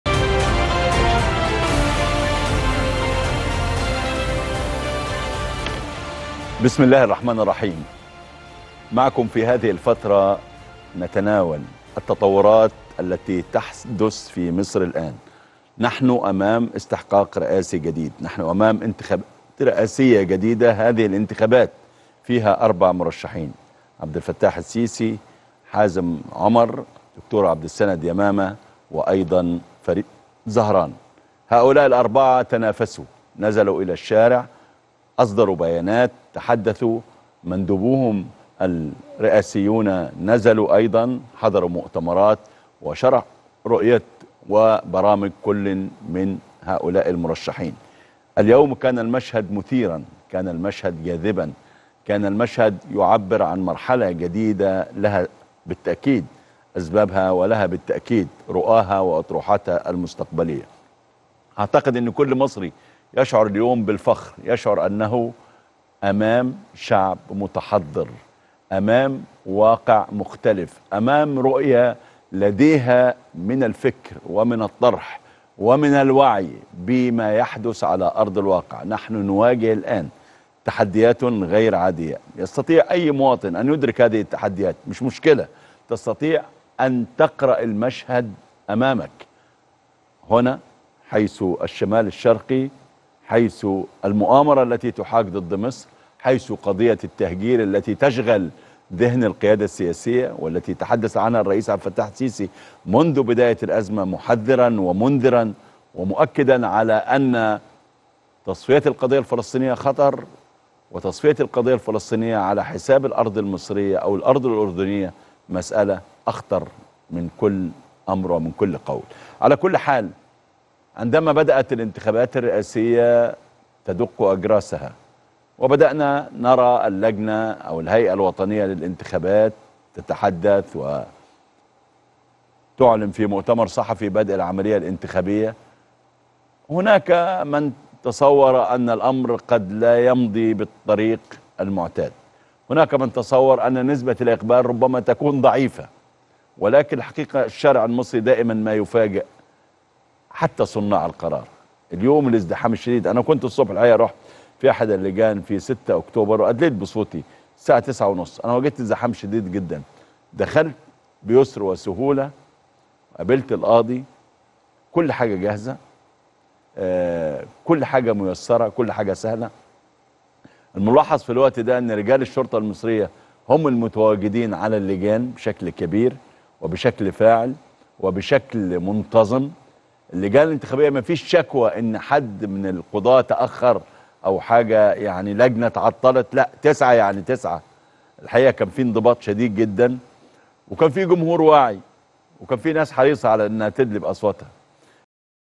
كل مصري يشعر اليوم بالفخر.. تعليق قوي من مصطفى بكري على مشهد انطلاق اليوم الأول من الانتخابات
تغطية خاصة لبدء الانتخابات الرئاسية المصرية 2024 وفيها  أهمية المشاركة السياسية وتحقيق التغيير الديمقراطي.بالإضافة إلى  لقاءات مع المرشحين والناخبين، واستعراض للمناظر من داخل مراكز الاقتراع، وتحليل لحملات الانتخابات والمواضيع السياسية الهامة المرتبطة بالانتخابات.